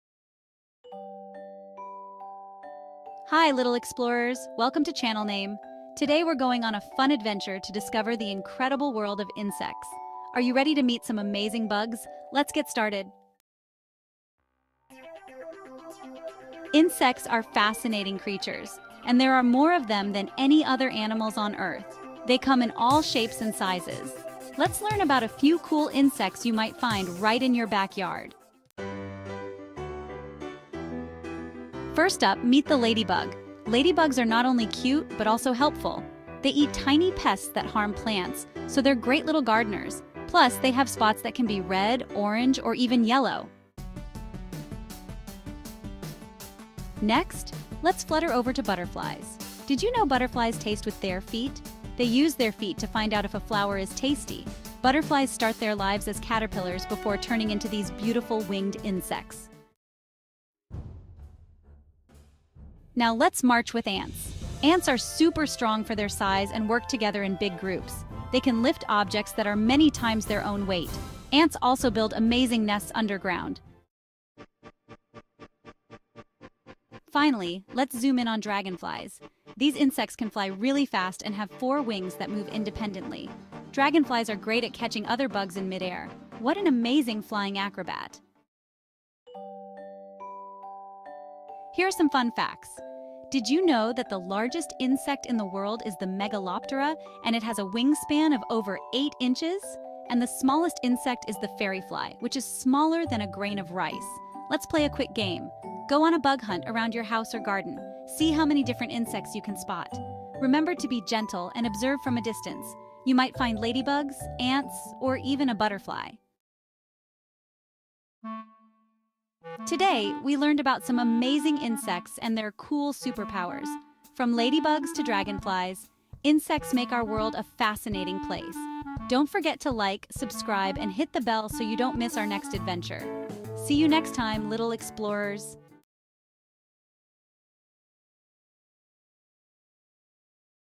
Incredible Insects!🐞Discover the Amazing Bugs Around Us!
Get ready to dive into the fascinating world of insects! 🌍🦋 From cute ladybugs that help in our gardens to speedy dragonflies with impressive flying skills, this adventure will introduce you to the incredible diversity of bugs. Discover how butterflies taste with their feet and learn about the amazing strength of ants. Meet the largest insect, the Megaloptera, and the tiniest fairyfly. Explore interesting insects in your backyard and appreciate their crucial roles in our ecosystem. Let’s embark on this exciting journey and uncover the secrets of our tiny, yet mighty, neighbors!